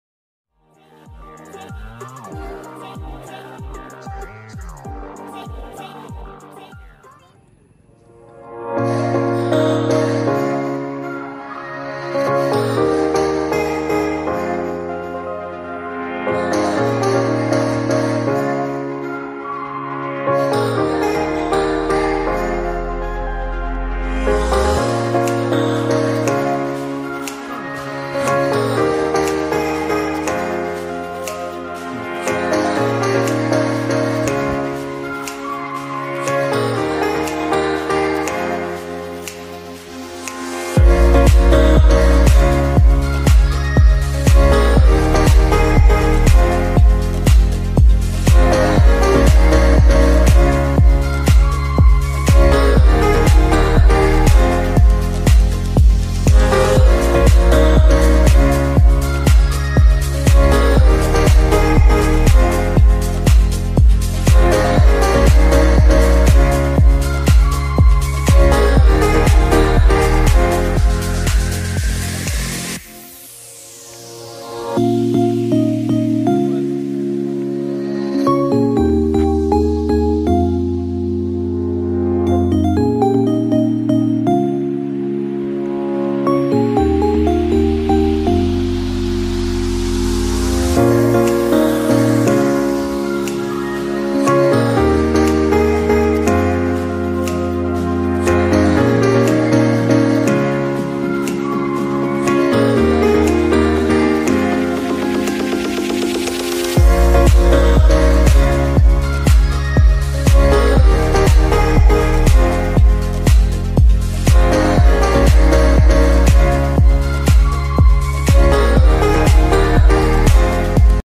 Long Jump//Synthetic track// calvarymount H S Ground// Calvary H S Ground//PhysicalTest Idukki2022//
Calvarymount Hs Ground
Idukki CPO physical test ground 2022.
Synthetic long jump pitch. Idukki